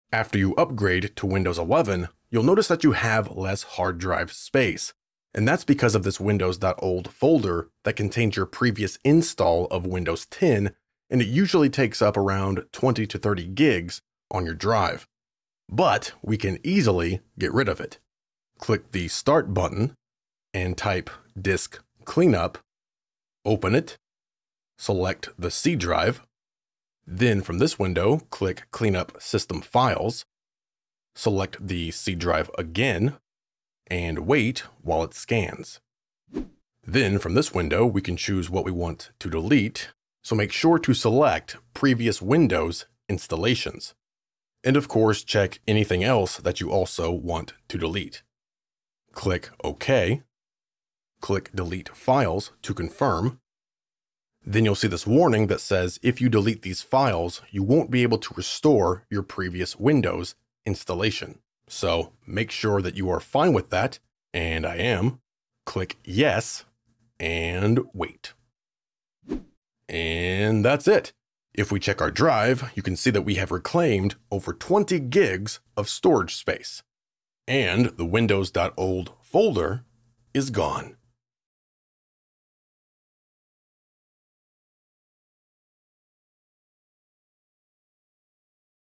Do THIS After Upgrading to Windows 11 (Get Drive Space Back) 👍
After you upgrade to Windows 11 you'll notice you have less hard drive space. This is because of the Windows.old folder that contains the previous installation of Windows.

But luckily, you can easily get rid of the Windows.old folder by using Disk Cleanup.

*Be aware that you should not delete the Windows.old folder until you are certain that Windows 11 is working properly. After deleting the folder, you will not be able to restore your system back to Windows 10 in case of a problem. This is not an issue for most people, just be aware.

After getting rid of the Windows.old folder, you will reclaim around 20-30GBs of drive space.

▶Our Links